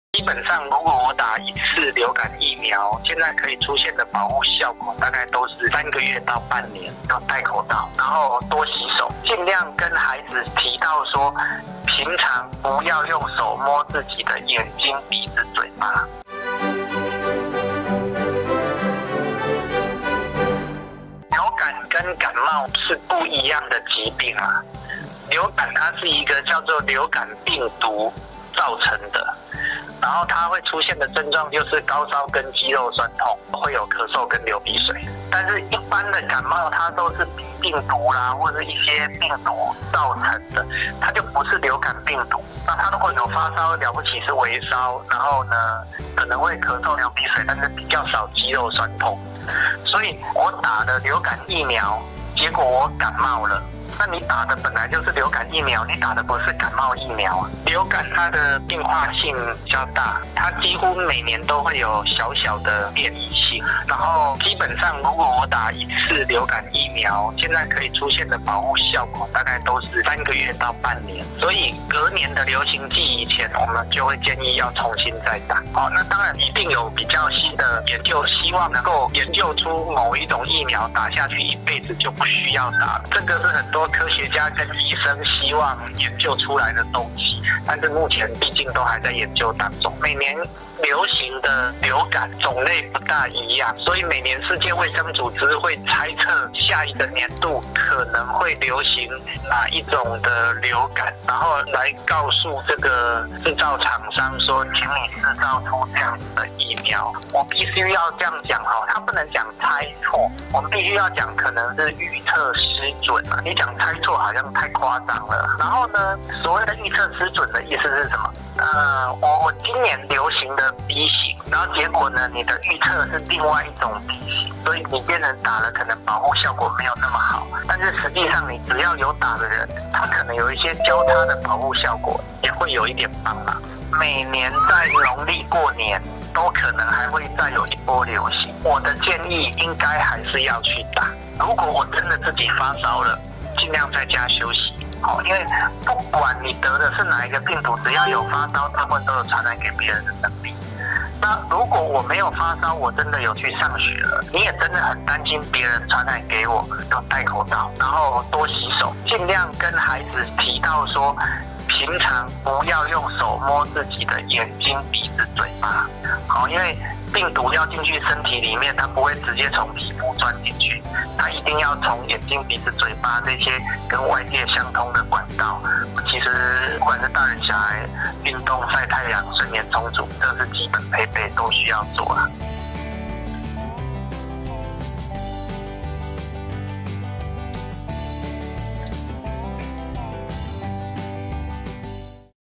【健康新聞快遞】流感疫苗沒效該不該打？柚子醫師教你如何自保抗流感
【健康新聞快遞】打完疫苗還是感冒？流感究竟該怎麼防範？

全台流感大流行，你和家裡的小朋友打了疫苗沒？
究竟在這一波疫苗「猜錯」的流感潮裡，
該如何自保才能保護家人和小朋友不受傳染？
今天小編電訪了小兒專科　陳木榮醫師，
就讓柚子醫生告訴你，究竟疫苗該不該打，
又該如何杜絕流感上身！

感謝 柚子醫師的育兒診療室 - Dr.陳木榮醫師